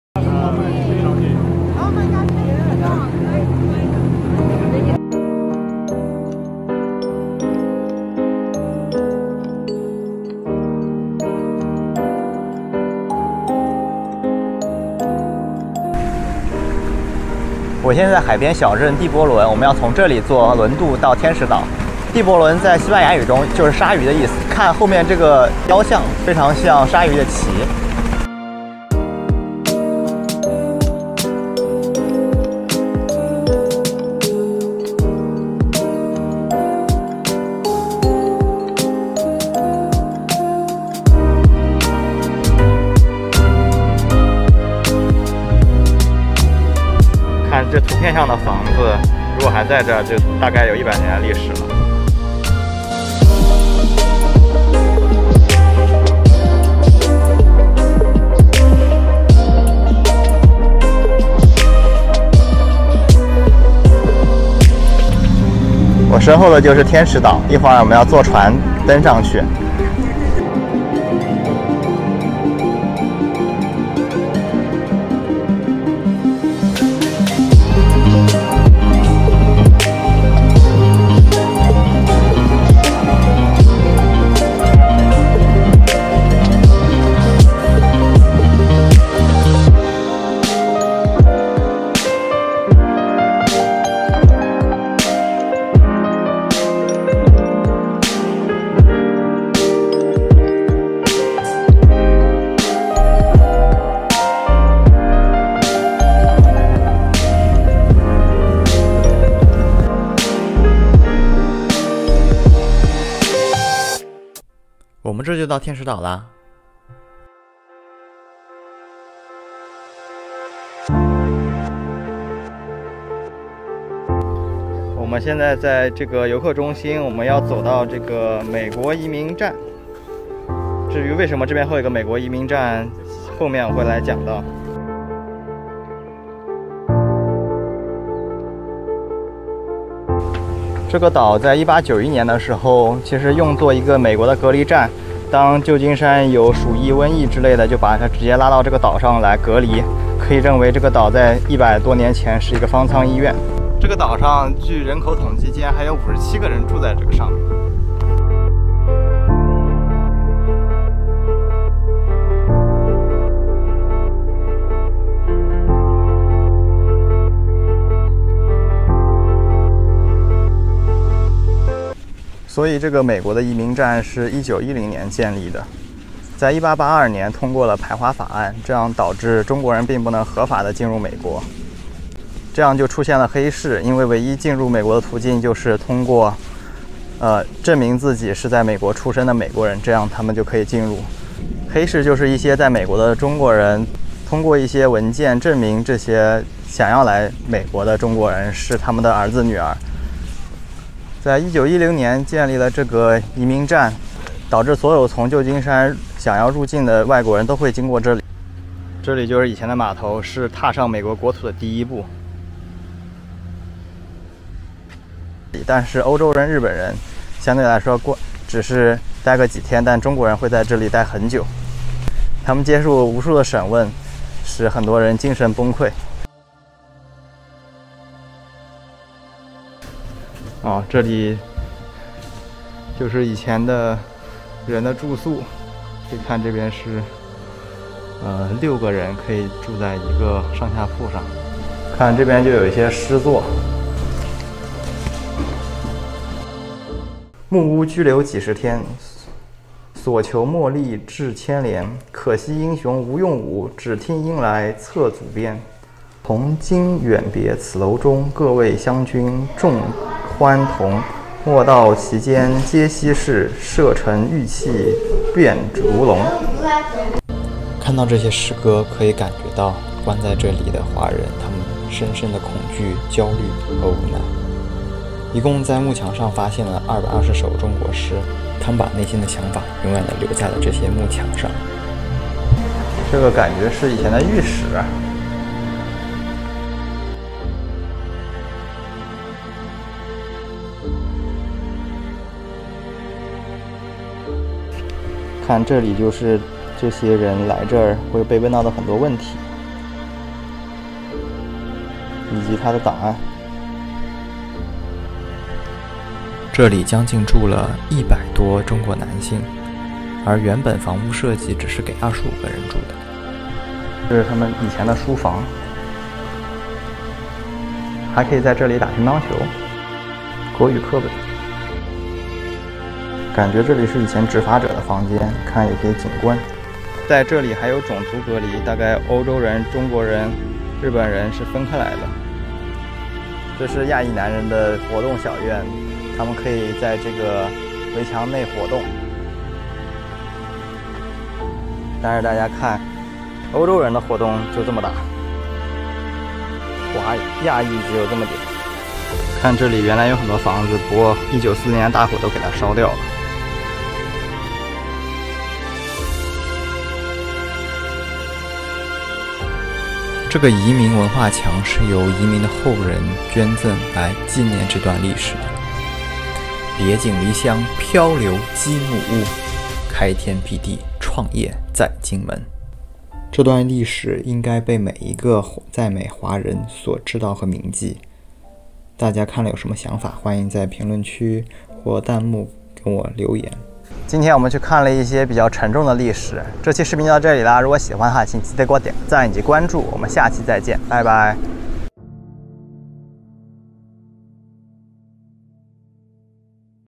为什么美国木墙上有中国的诗歌 | 天使岛
带大家去看看旧金山旁边的小岛天使岛，曾经的美国移民站也在那里。